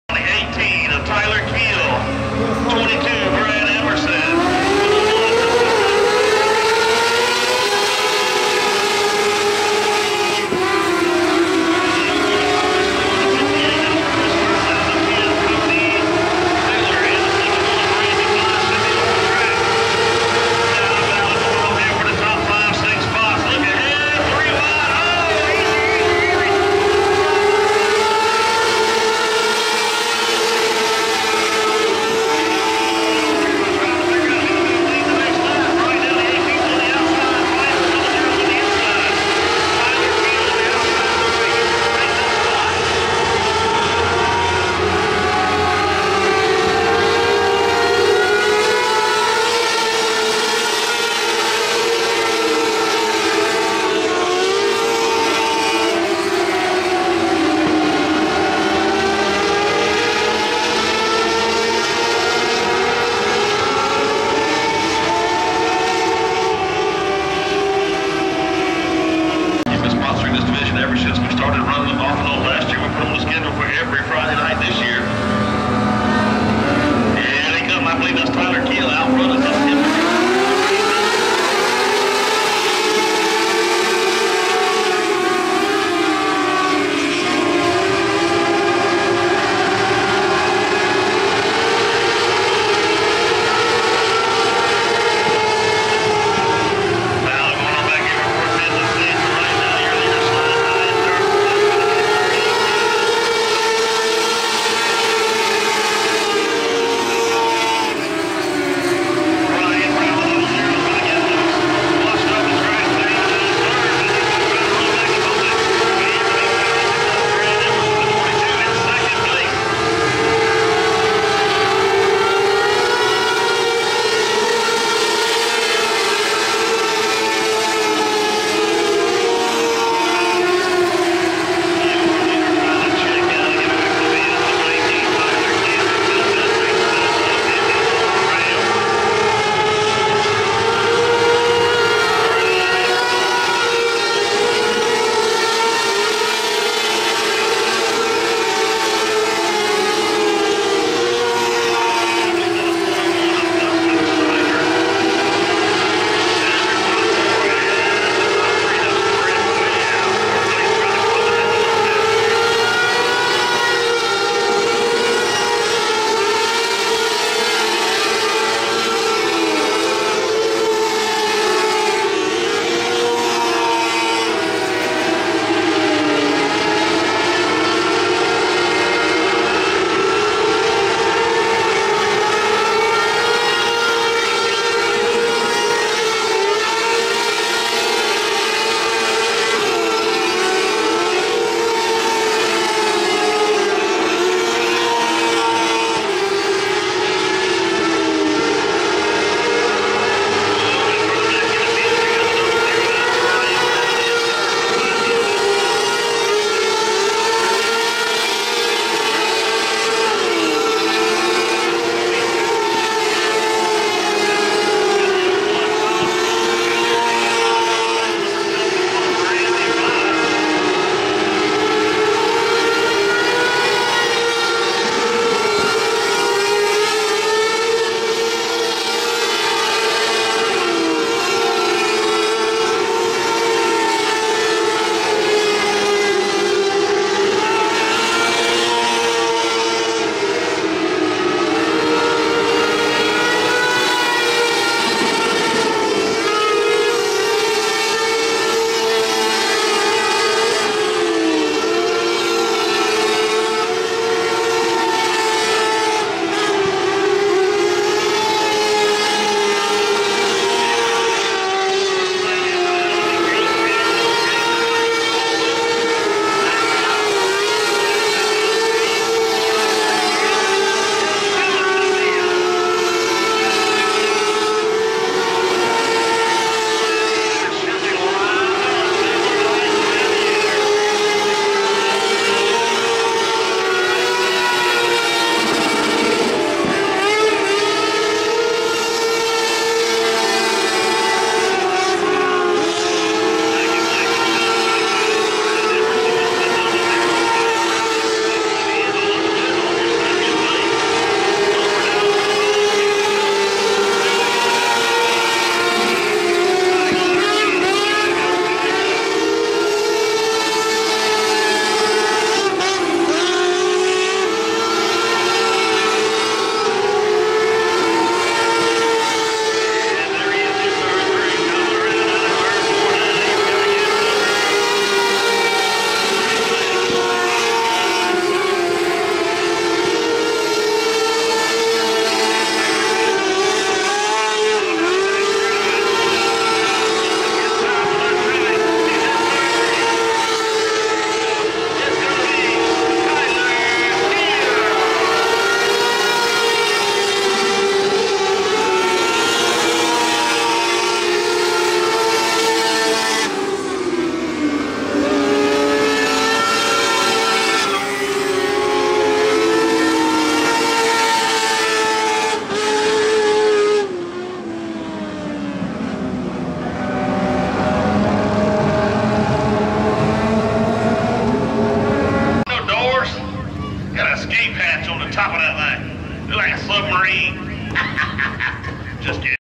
09/04/20 Mini Late Model Feature Race
Congratulations 1st place #18 Tyler Keel, 2nd Place #22  Brad Amerson, 3rd Place #00 Ryan Brown